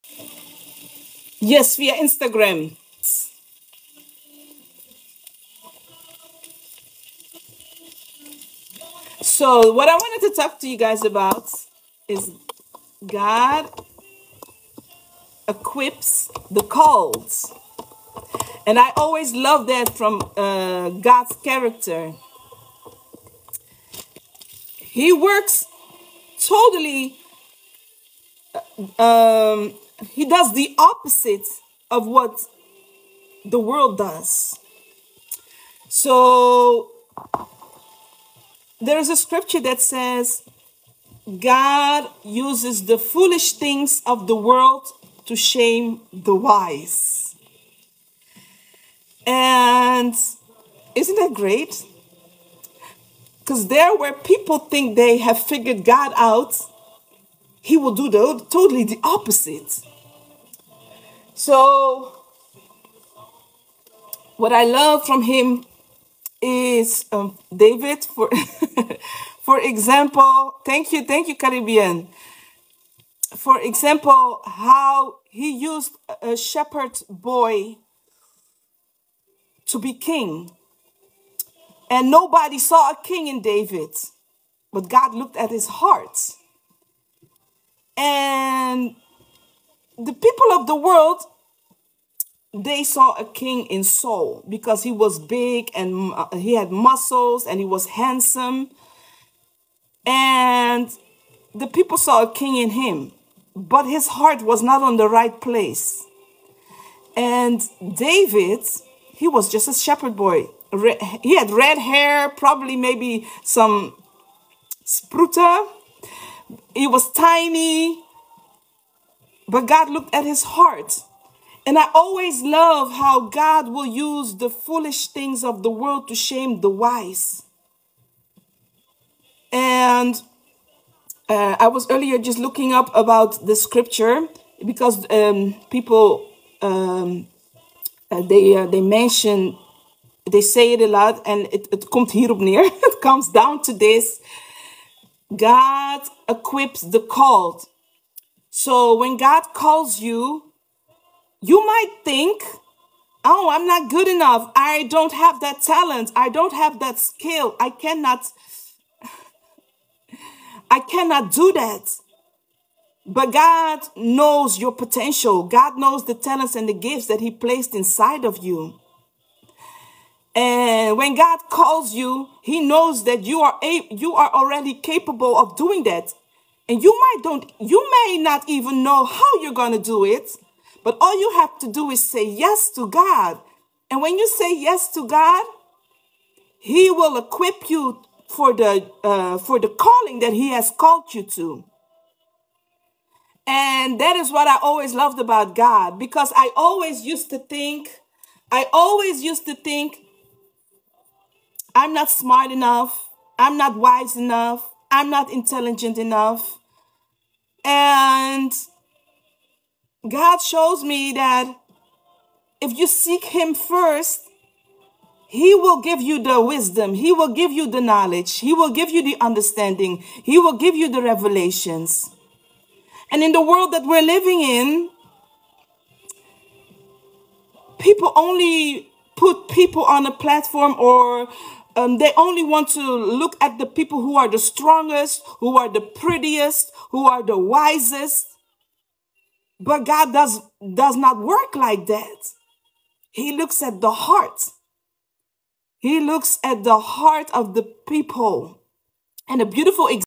God Equips the Called!
When God calls you, answer the call. He will equip you for the task!
Hebrews 13:21

What has God called you to do? 🙏🏾❤️